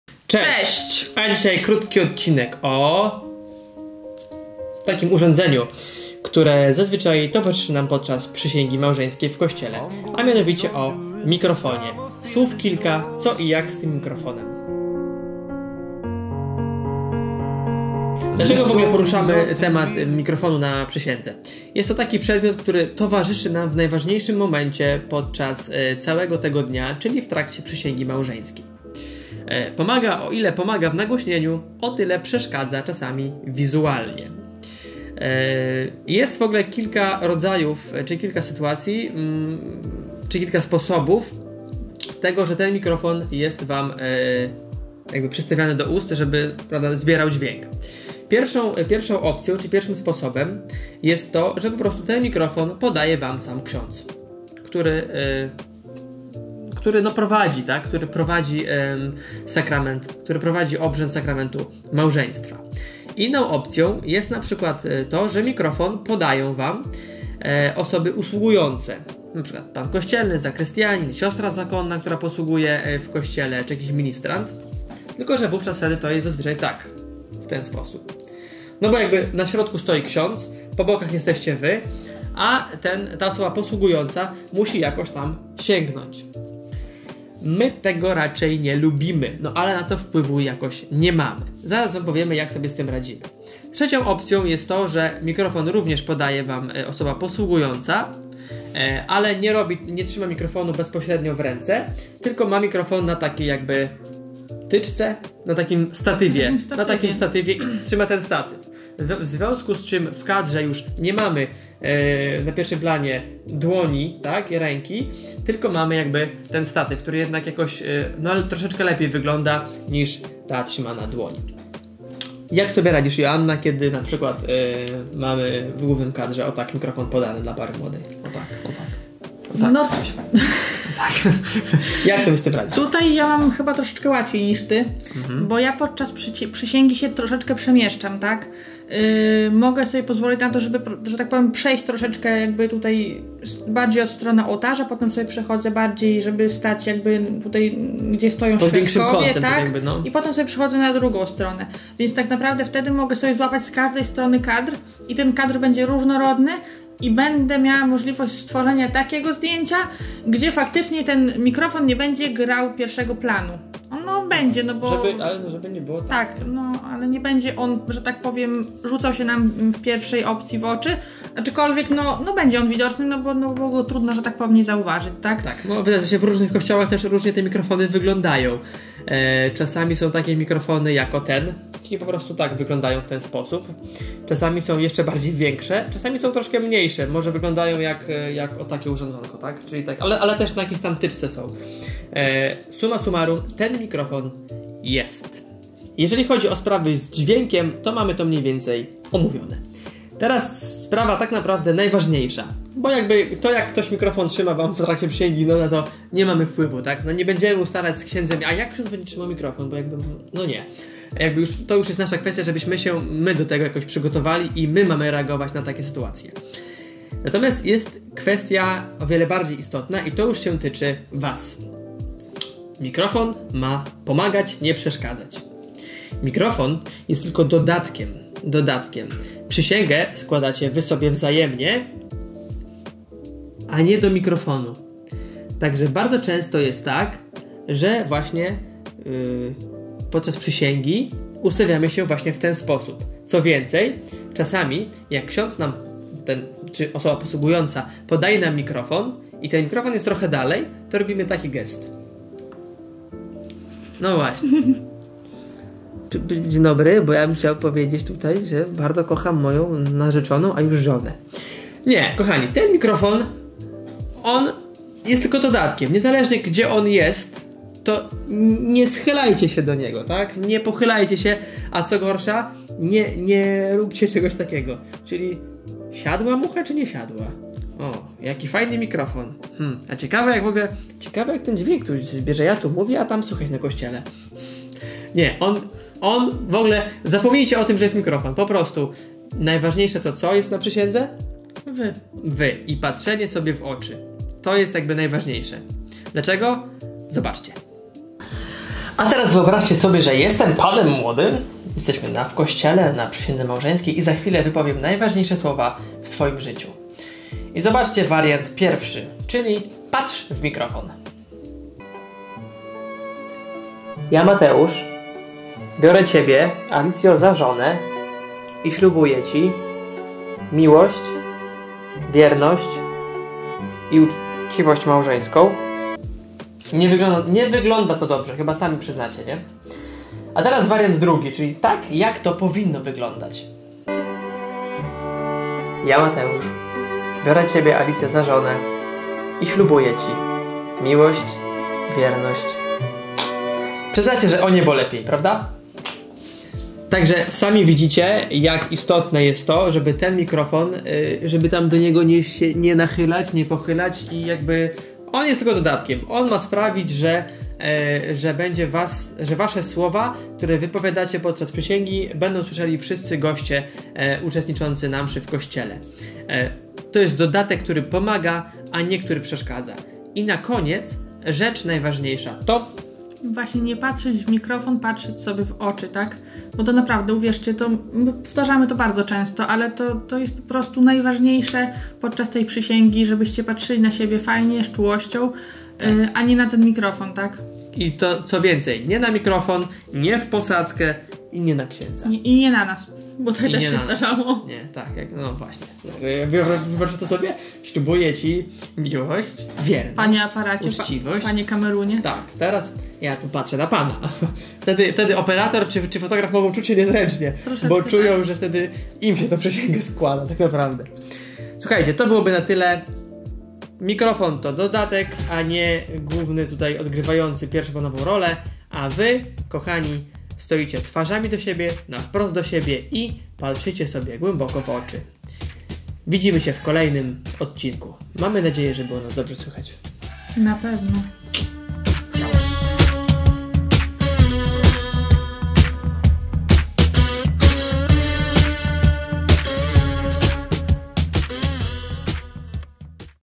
I czar prysł... 🎤🎛⛪️ [przysięga, kościół]
Co zrobić, aby mikrofon nie był przedmiotem, który psuje cały czar przysięgi małżeńskiej?